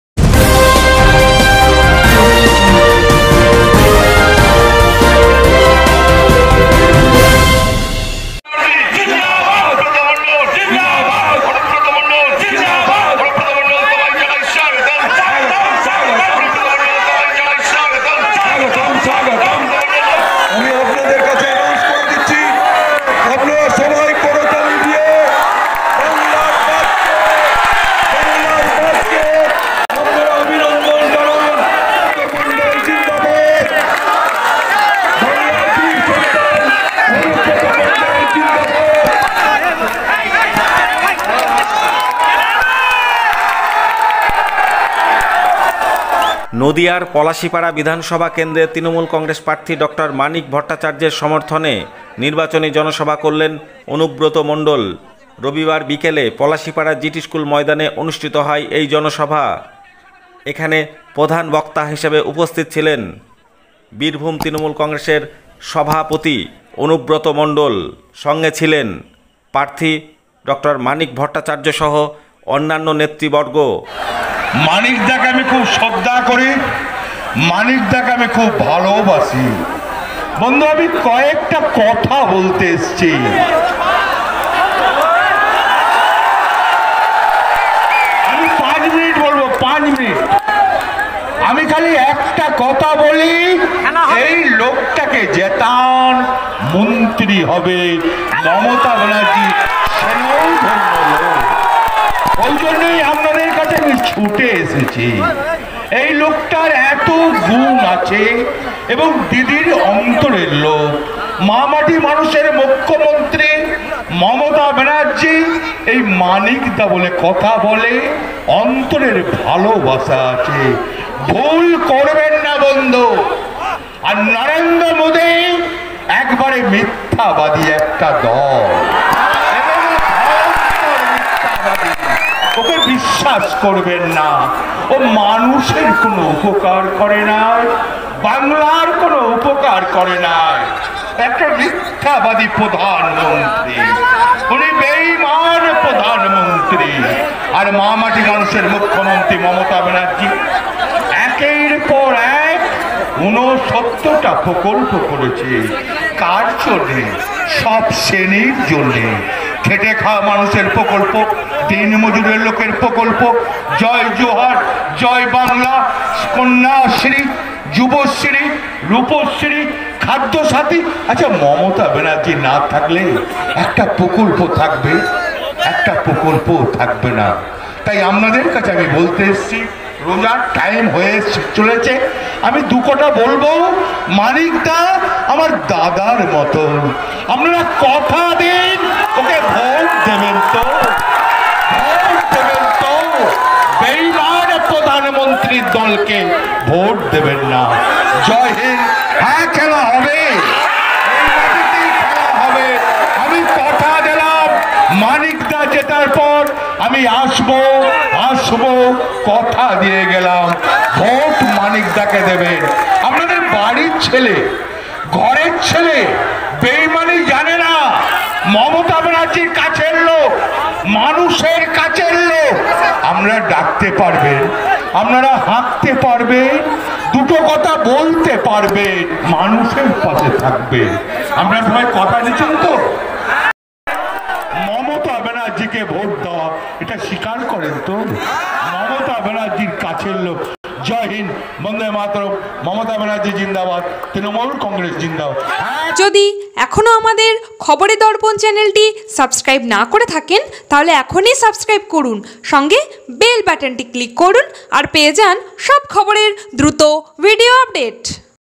পলাশীপাড়া কেন্দ্রের তৃনমূল প্রার্থীর সমর্থনে জনসভা থেকে কি বললেন অনুব্রত মণ্ডল দেখুন
নদীয়ার পলাশীপাড়া বিধানসভার তৃনমূল প্রার্থী ডঃ মানিক ভট্টাচার্যের সমর্থনে পলাশীপাড়া জিটি স্কূল ময়দানে অনুষ্ঠিত জনসভায় কি বললেন অনুব্রত মণ্ডল দেখুন।
পলাশীপাড়া# নদীয়া